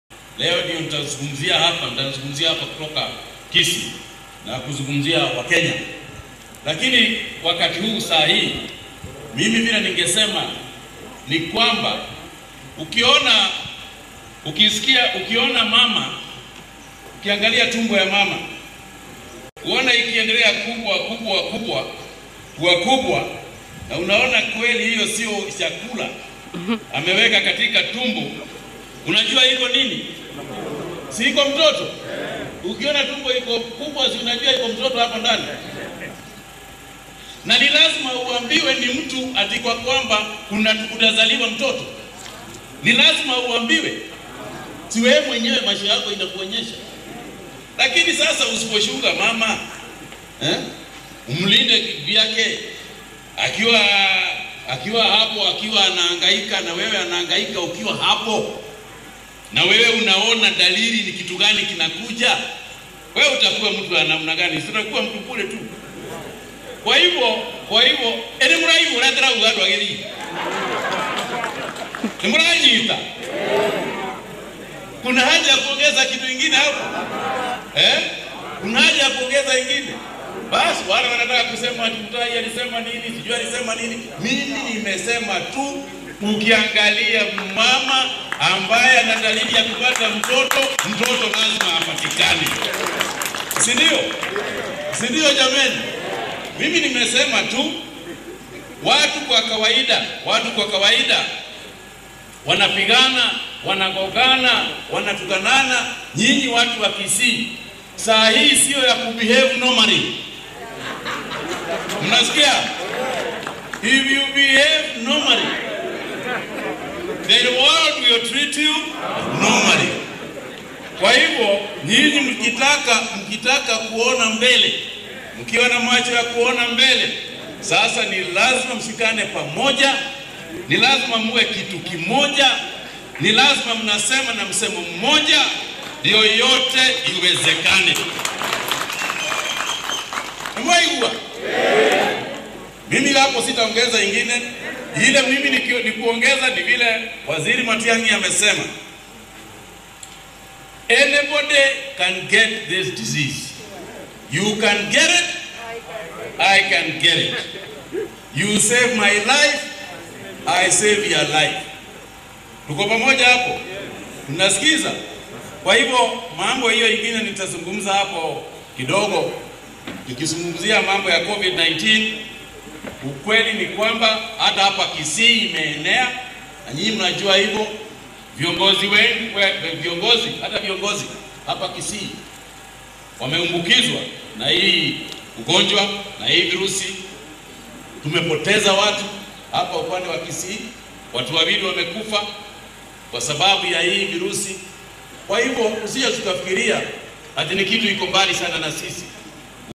Did CS Kagwe campaign for CS Matiang'i 2022 in Kisii? - Ruto Camp in Panic
Cabinet secretaries Mutahi Kagwe and Fred Matiang'i were in Kisii on Friday where CS Kagwe made statements that people are interpreting as supporting CS Matiang'i for top seat in 2022 elections. If this is the case, DP Ruto's tangatanga team should be worried by this competition.